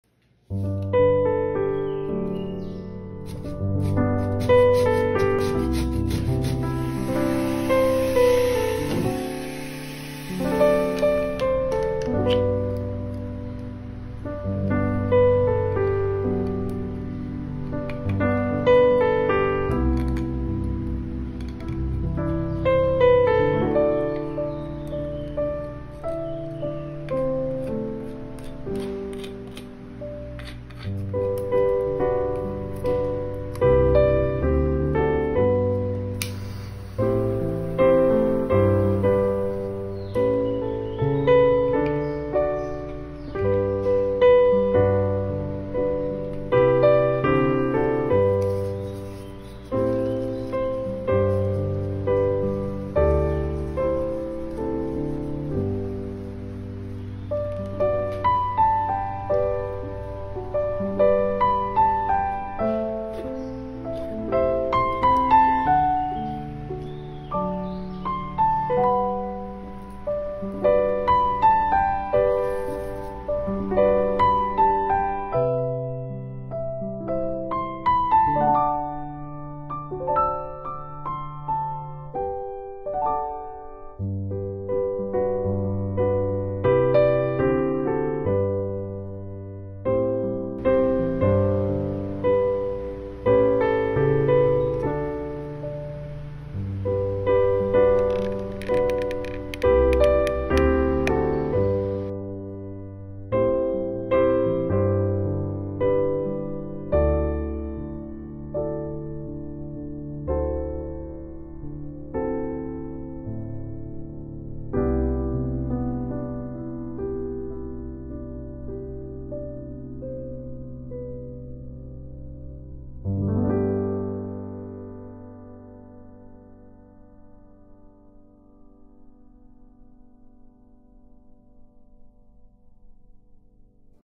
Modification to Lensbong Sony H400 for mobile macro photography using smartphone.
This video show how I cut short the macro lens housing so that when mounted in front of the lens of your mobile phone, you don’t need to zoom/crop too much to avoid the vignettes at the edge of the photo. This will improve the image quality.